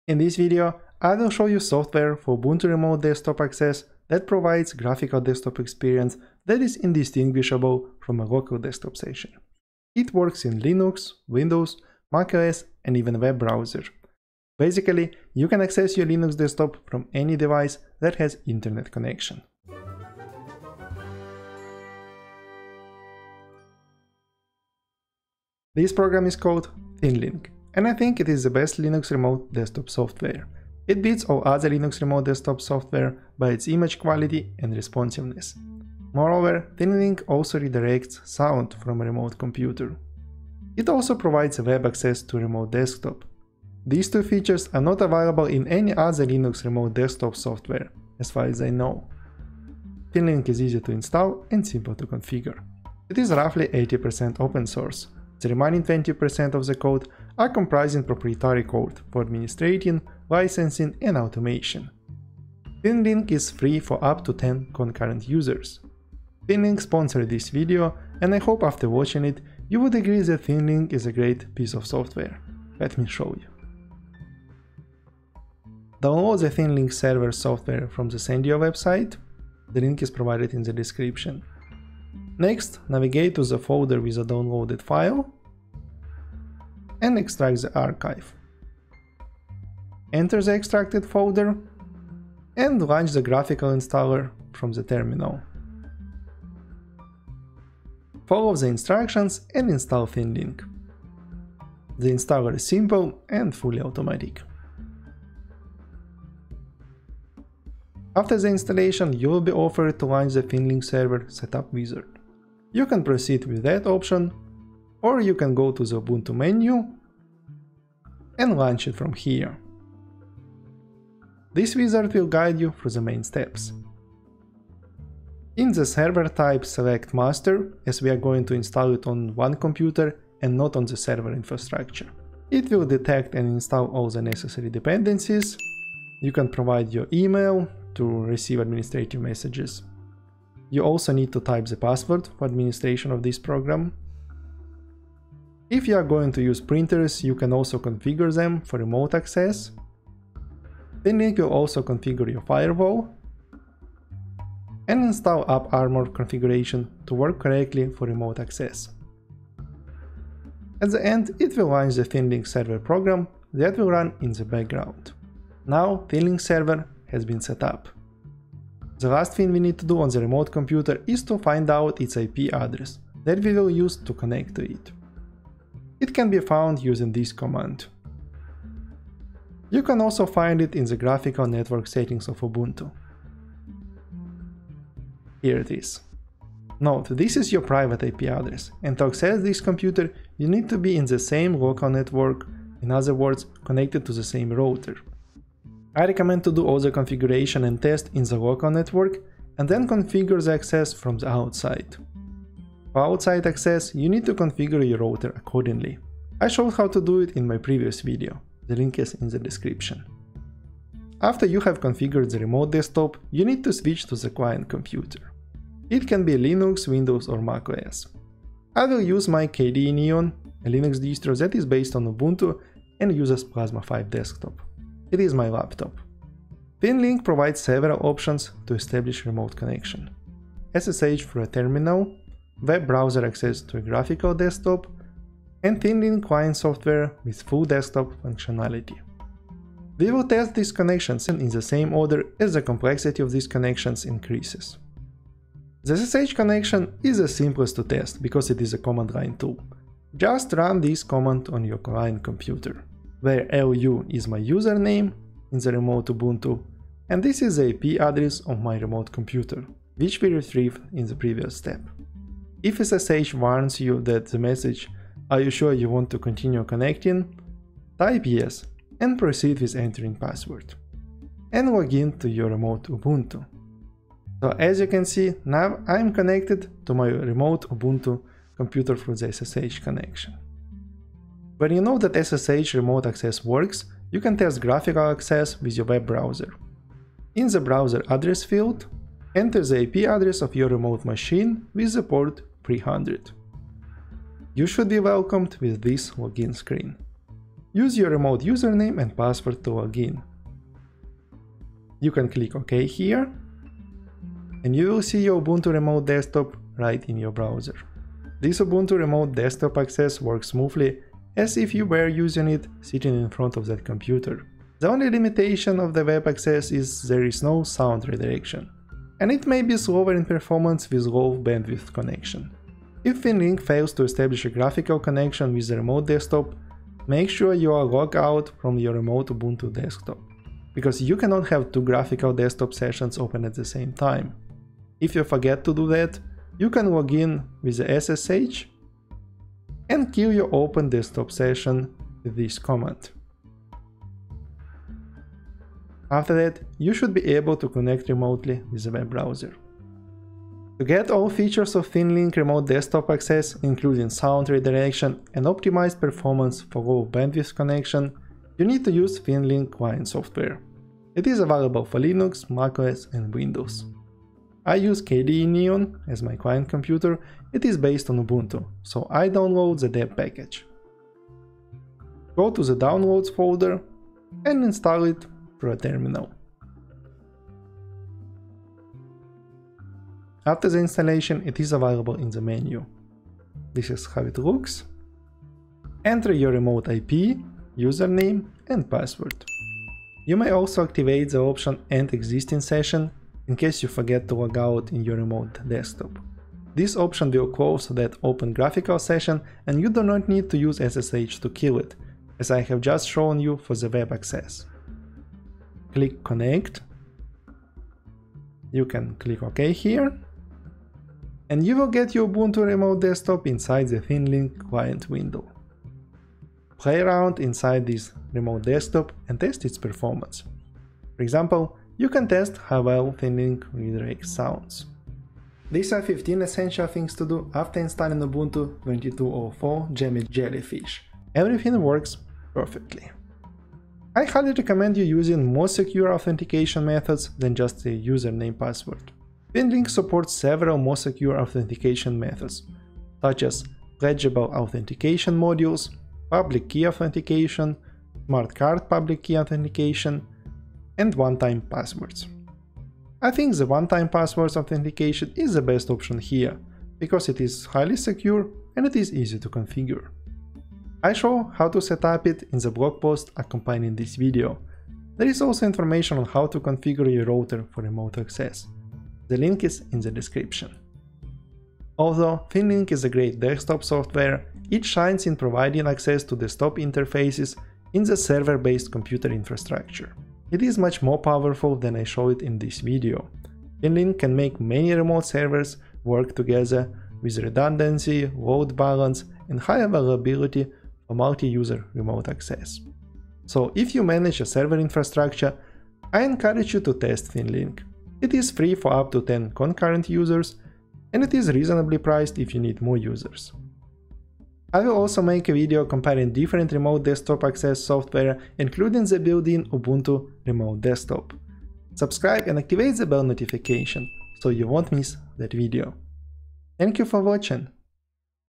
Ubuntu Remote Desktop Access with ThinLinc
ThinLinc is the best Remote Desktop Access software that provides graphical desktop experience that is indistinguishable from a local desktop session. It works in Linux, Windows, macOS, and even a web browser. Basically, you can access your Linux desktop from any device that has an internet connection.

More detailed tutorial including terminal commands, enhanced security settings, router configuration

ThinLinc sponsored this video and I hope after watching it you would agree that ThinLinc is a great piece of software.

My other hardware

BTC: 1CfCLTYCqqDfYbngcvQtEav3n7SMZihfSS

Timecodes:
0:00 Overview
0:25 ThinLinc is the best Remote Desktop Access software
01:14 Install ThinLinc
01:48 Configure ThinLinc
02:48 Get your IP address
03:31 Client computer options
04:05 SSH connection
04:41 Web browser access
05:50 ThinLinc Client Software
07:13 Enhanced Security
07:51 Server-based computing
08:24 Comparing different Remote Desktop Access software